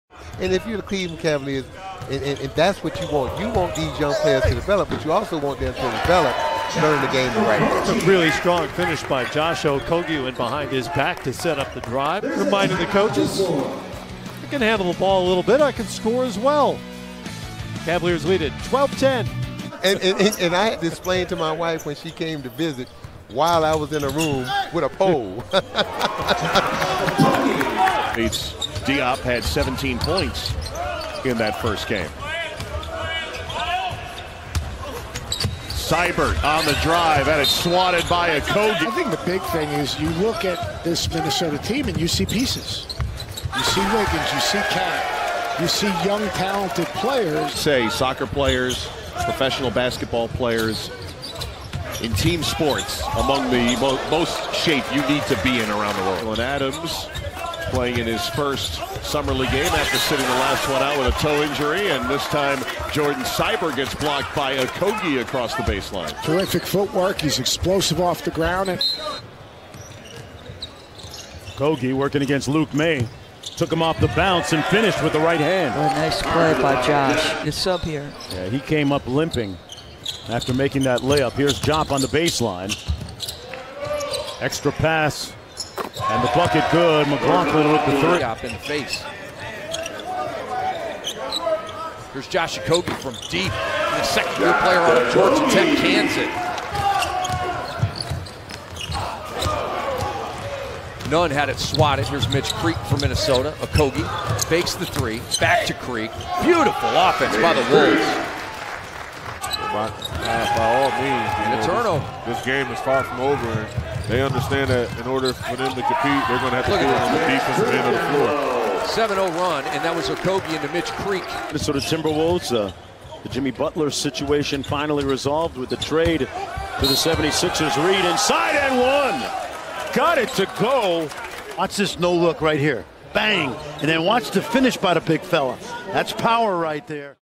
Josh Okogie 2019 Summer League Highlights
The top plays from Josh Okogie at 2019 Summer League.